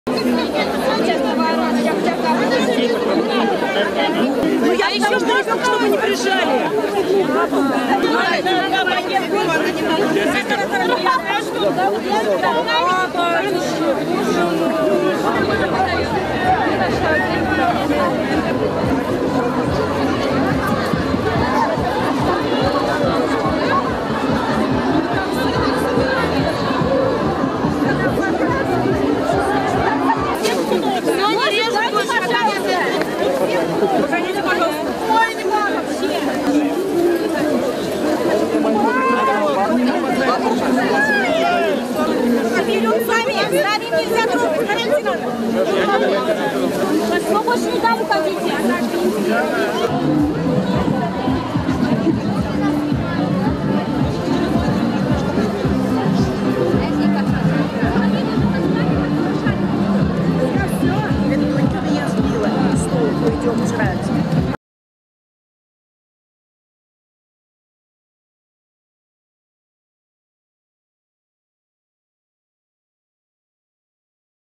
Битва за чак-чак в Нижнекамске
Почти 60 тыс. человек, по данным УВД города, побывали в субботу на Сабантуе в Нижнекамске. Зрители, среди которых оказалось немало VIP’ов, посмотрели театрализованное представление и устроили битву за национальное лакомство. Корреспондент «БИЗНЕС Online» перемещалась по территории майдана вместе с руководителем татарстанского парламента.

Видео: Антонина Никипчук
Подготовил: Илья Савельев